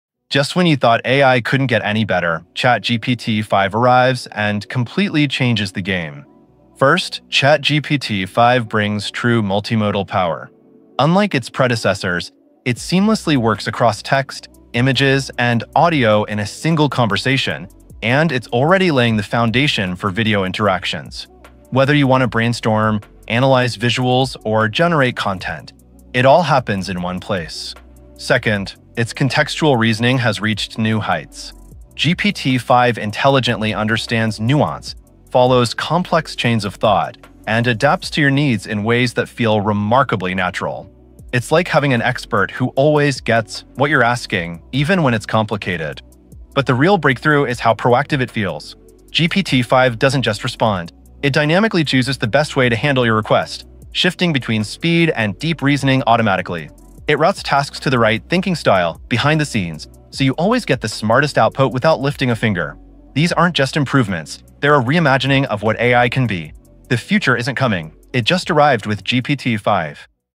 What’s New in ChatGPT-5?
Discover the groundbreaking advancements in ChatGPT-5, the latest revolutionary AI language model from OpenAI that's transforming how we interact with artificial intelligence. This next-generation model boasts unprecedented language comprehension, enhanced reasoning capabilities, and remarkable contextual awareness that far surpasses its predecessors. ChatGPT-5 delivers faster response times, improved accuracy, and a deeper understanding of nuanced queries, making it the most powerful AI assistant available today for both casual users and professionals seeking cutting-edge AI solutions.

We only recommend products and services that we have personally used and believe will add value to our audience.